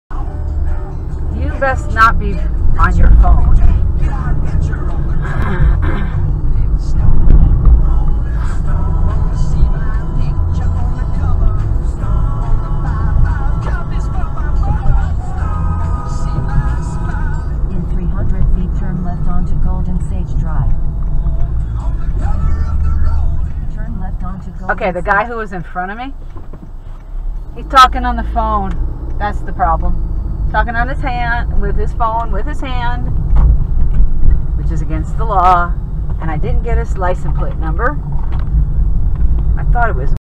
2019 12 27 Talking on phone while holding the phone
Wish I had gotten this license plate number.  I suspected this driver was talking on the phone while holding the phone which is illegal.  Once I passed him to get into the left turn at a road off Cochran's Crossing Dr, I saw that yes, I was correct in thinking that.  This was at 4:18 p.m. on 12/27/2019